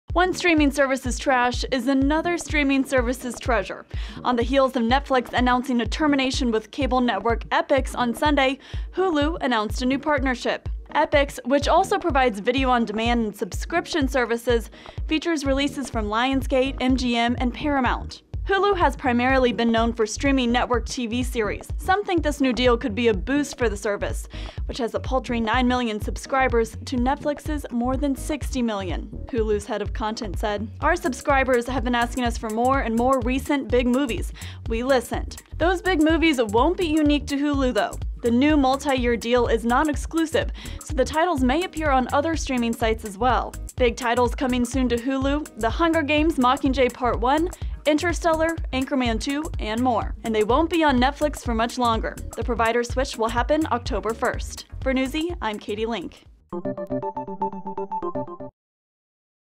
Netflix Drops Epix Movies, Hulu Picks Them Up - Newsy
After Netflix dropped Epix movies, Hulu announced a deal with the company.